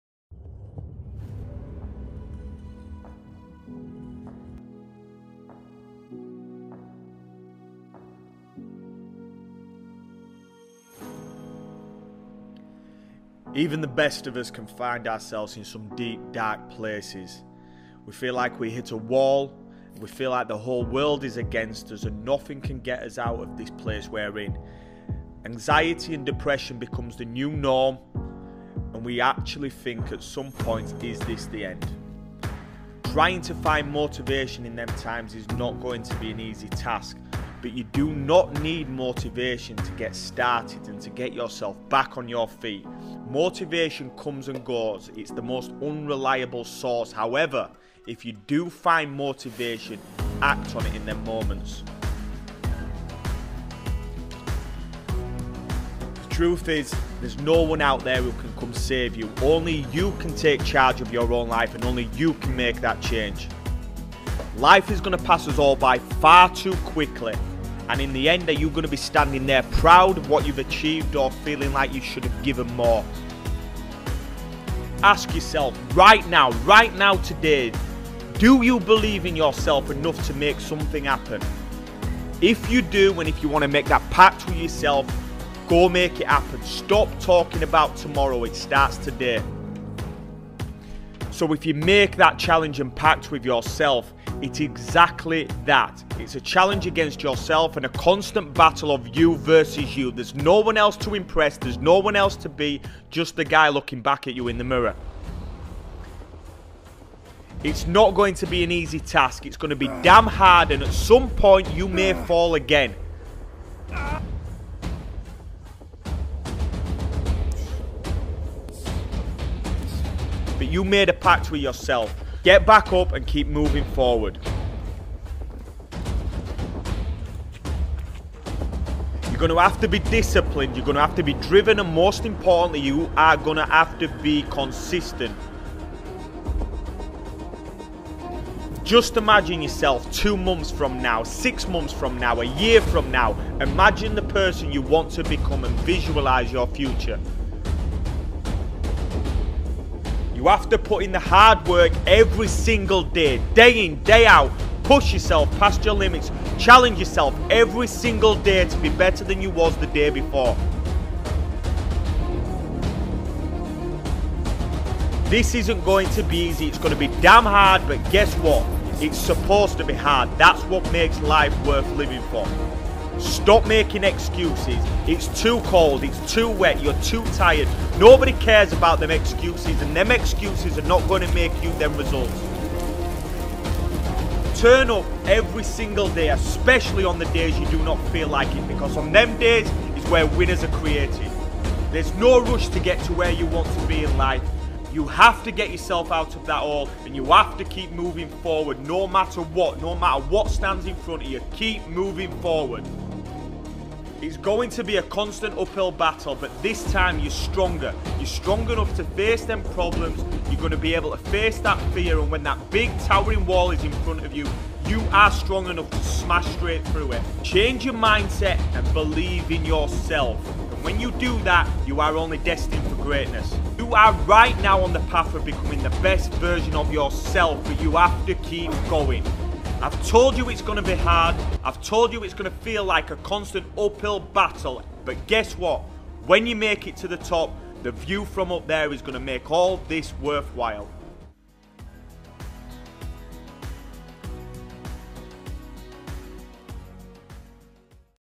KEEP MOVING FORWARD - RFS fitness - motivational speech
The best of us can hit rock bottom but we are all capable of getting back on our feet and getting back on top